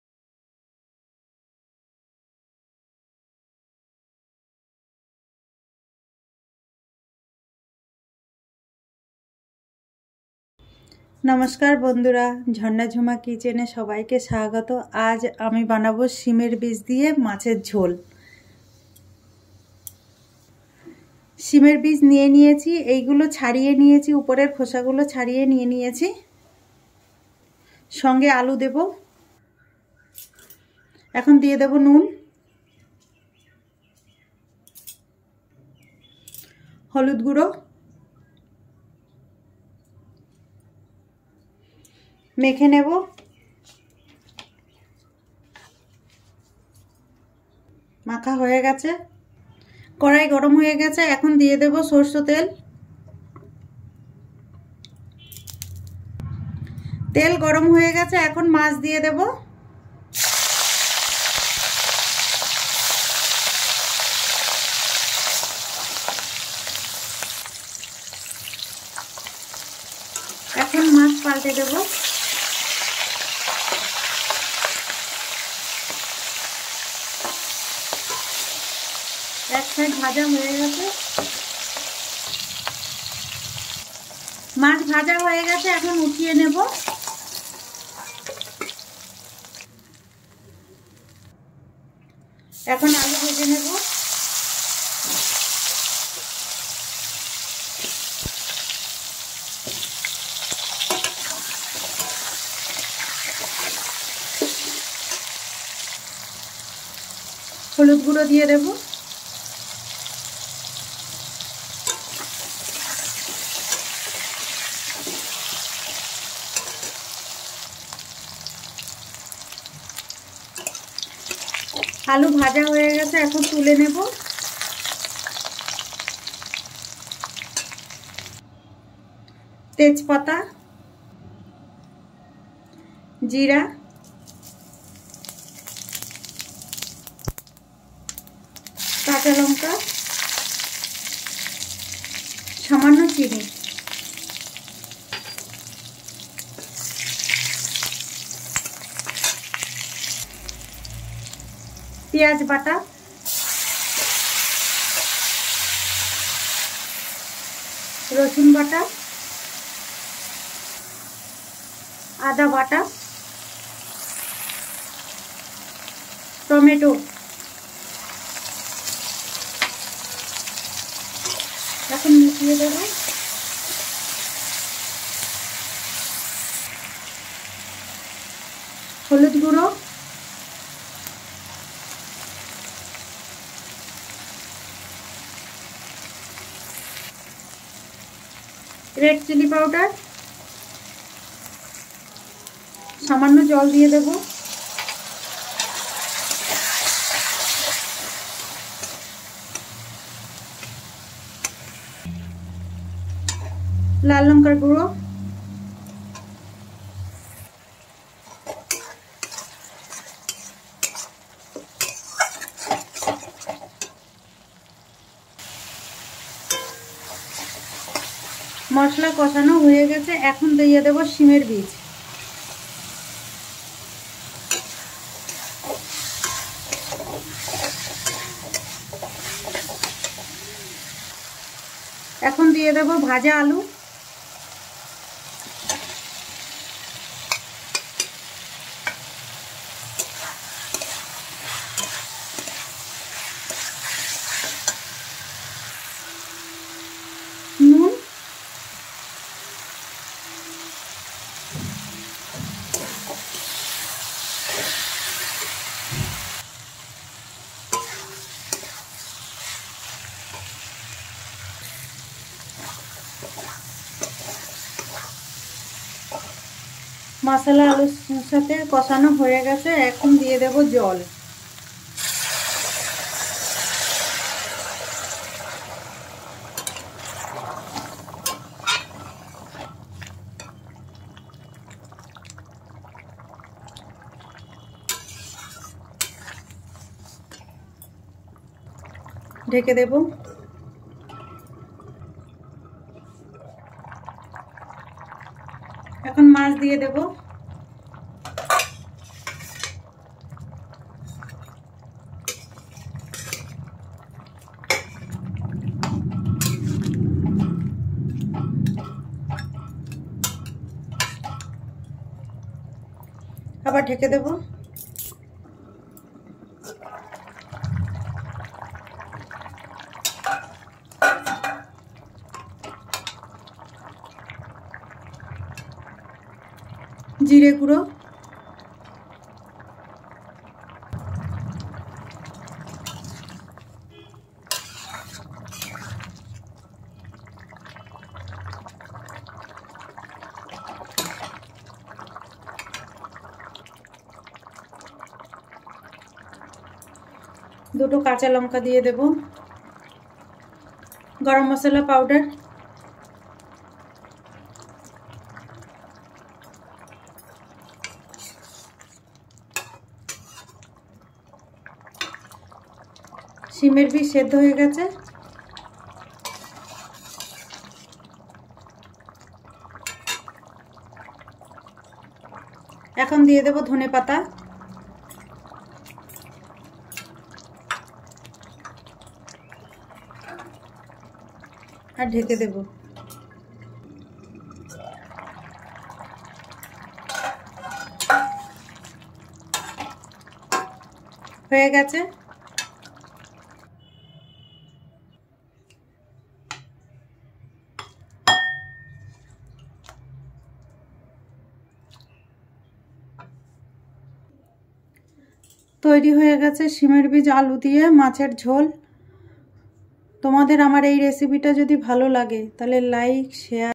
Seem Seeds with Fish Curry |Beans seeds diye macher jhol |jharna juma kitchen.
Hi!
In this video Beans seeds diye macher jhol.
I'm jharna Das kirtania from West Bengal.